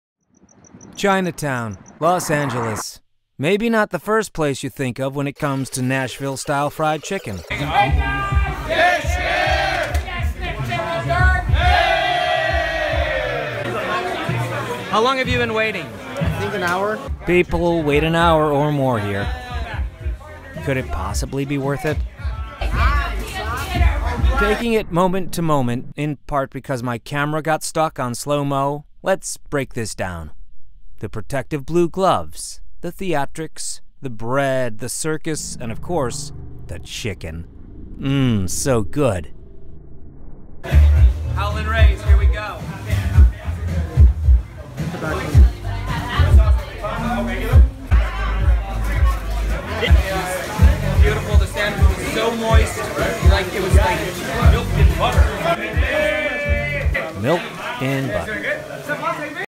Howlin' Rays
Fantastic fried chicken sandwich and a lively show by the crew at the boisterous and fun Howlin' Rays. Prepare to wait in line, or time-it-right to get in sooner. Spiciness is up to your own discretion - note the blue gloves !